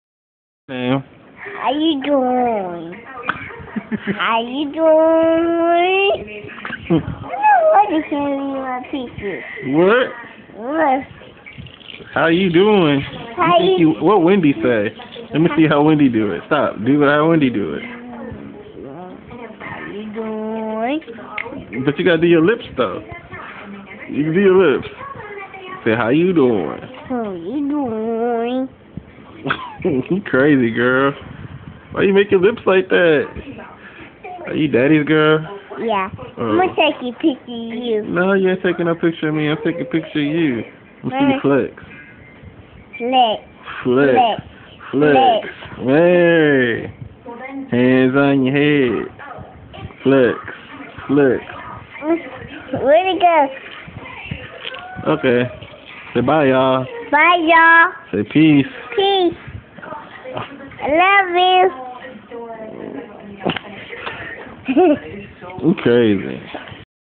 Skai the texas star doing wendy lips...
Skai 2 yrs old doing the wendy lips and the flex acting crazy